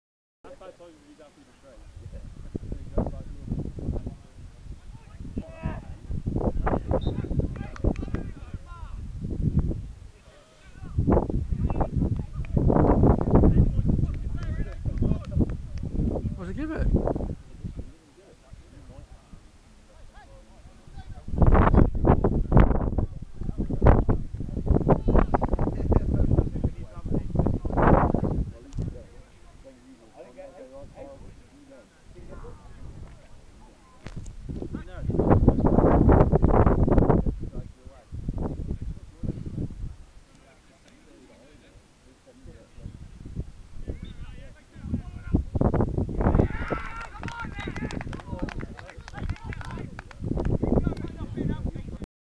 C.C,F.A Bond Timber Junior Cup 1st Round Game, Boscastle 2 v 6 St Stephen
match highlights Boscastle get a penalty as Curtis Dalby is judged by the  referee of tackling from behind a Boscastle player just inside the penalty area even the Boscastle players seem suprised at the decision.Rob Heal scores from the penalty so 1 - 2 on 3 minutes.